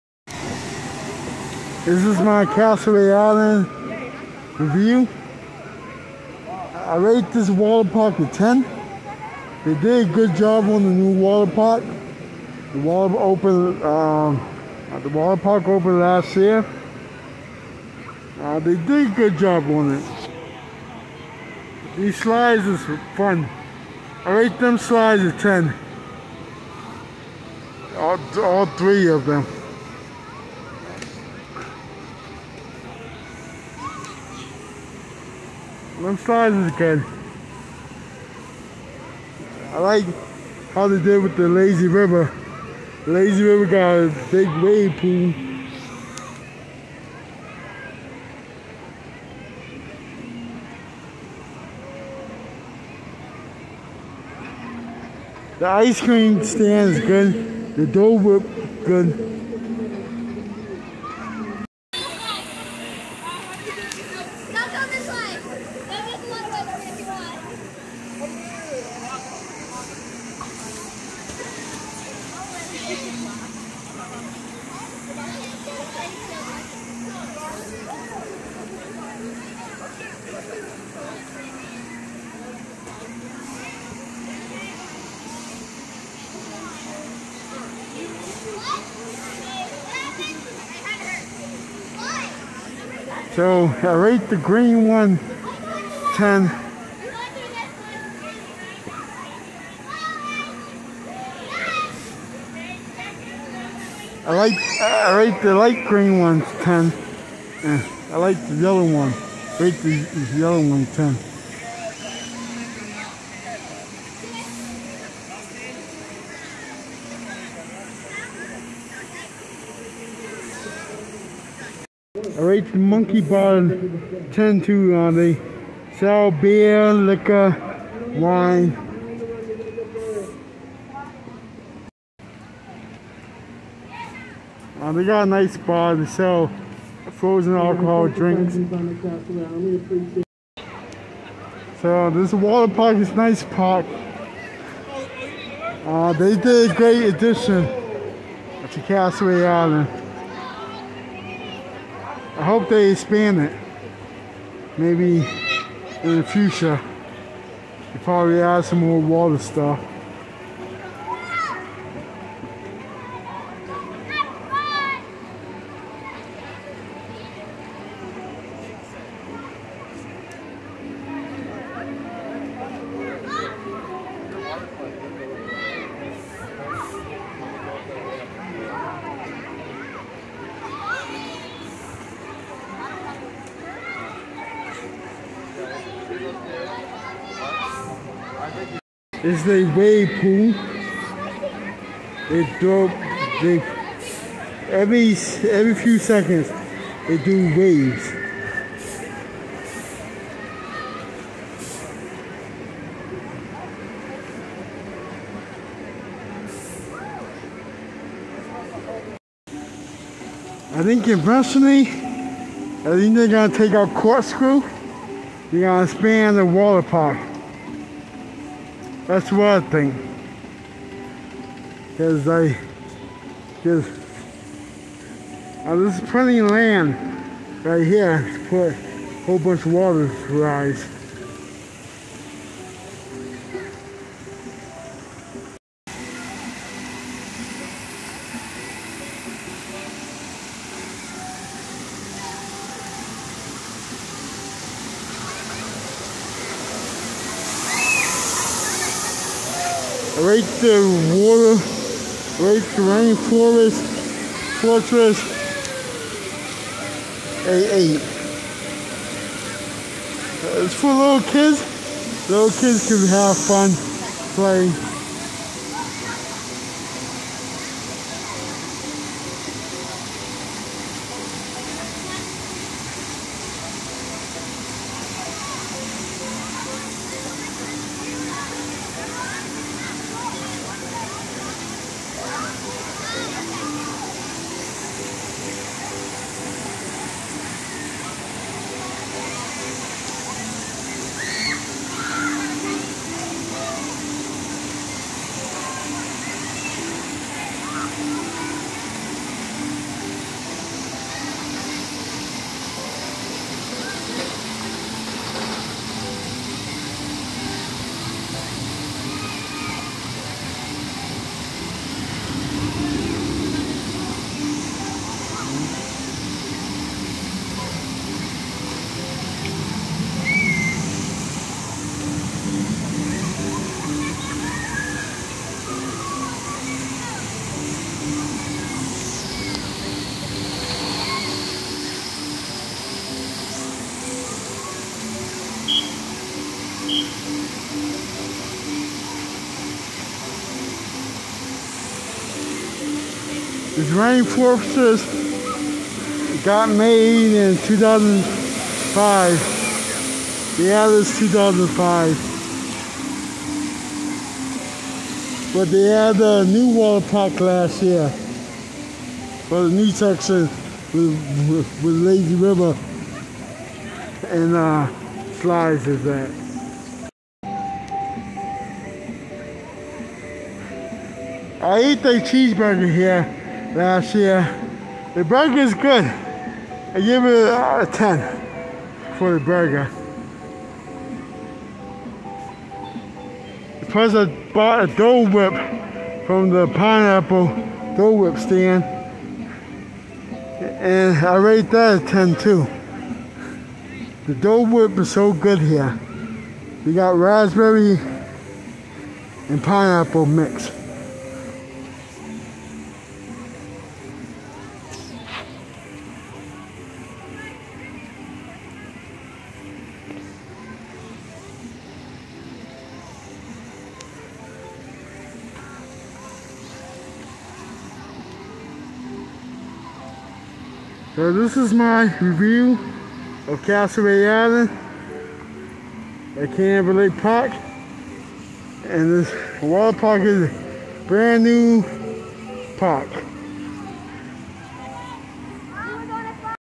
My Review of The New Castaway Island Water Park at Canobie Lake Park
My Review of The New Castaway Island Water Park at Canobie Lake Park That Got built  in 2018 and Open In 2019